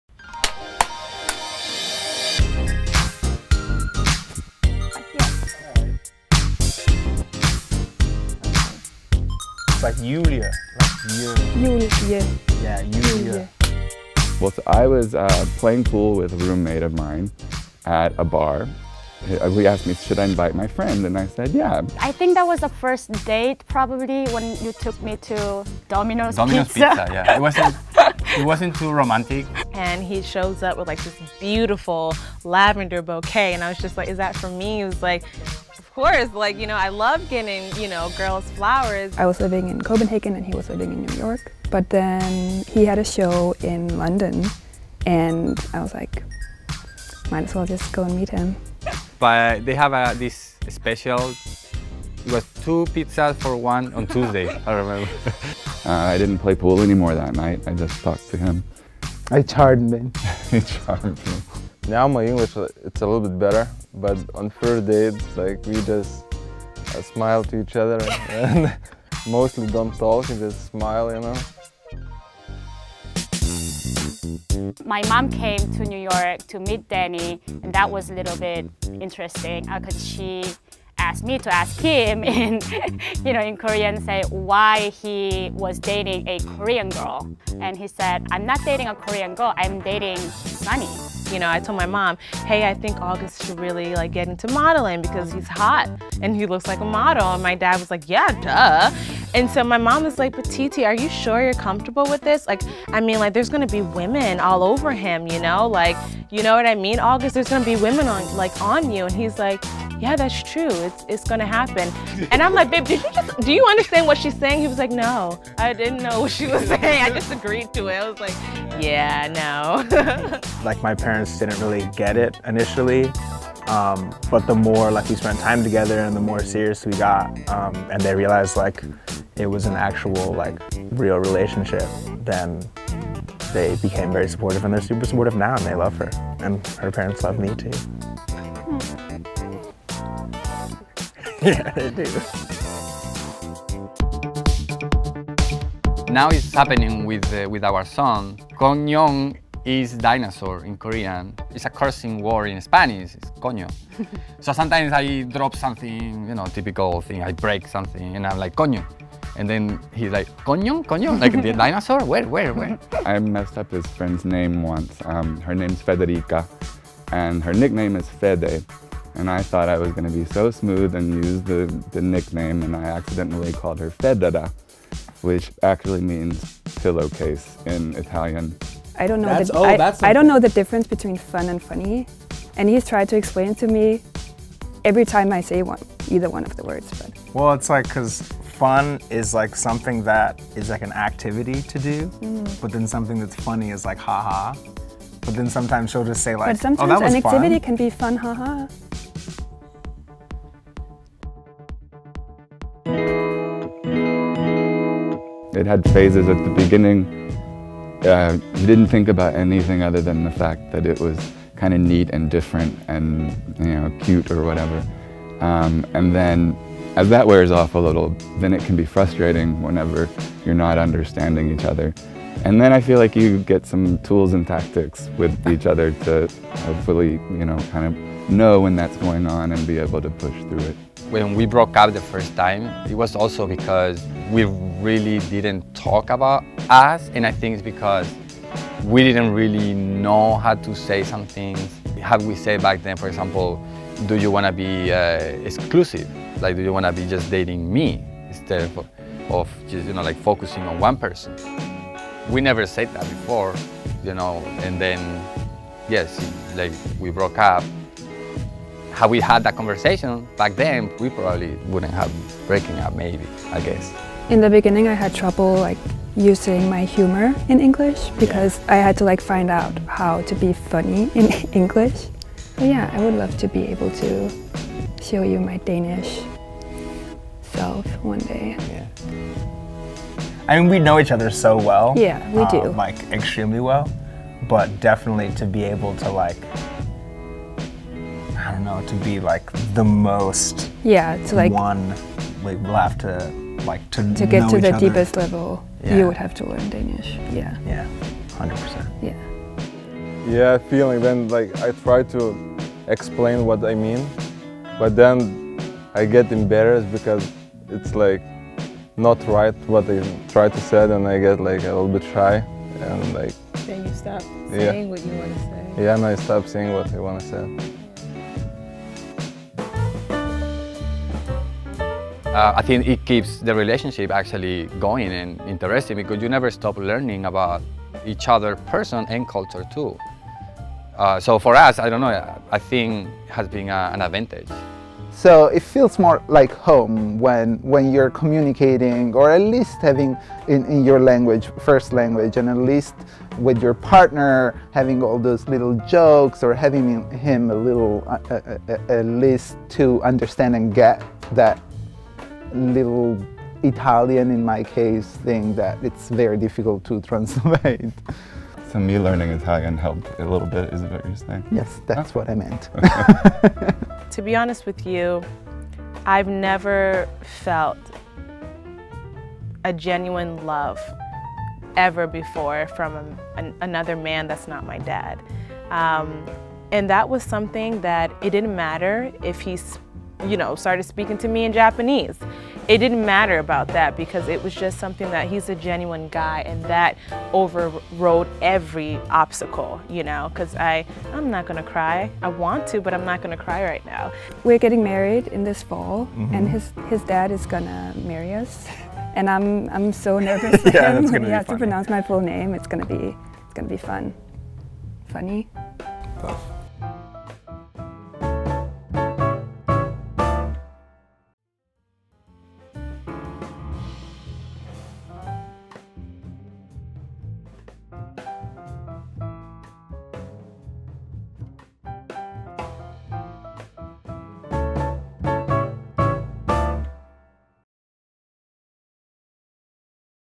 Couples Talk Language and Love
We interviewed four couples with different first languages about how hard it is to love someone when you don’t always understand them. Produced and directed by filmmakers Brandon and Skyler Gross, creators of the short film “On My Way Out”.

Check out Brandon and Skyler’s film, selected for TIFF and the SXSW Film Festival – we’re fans!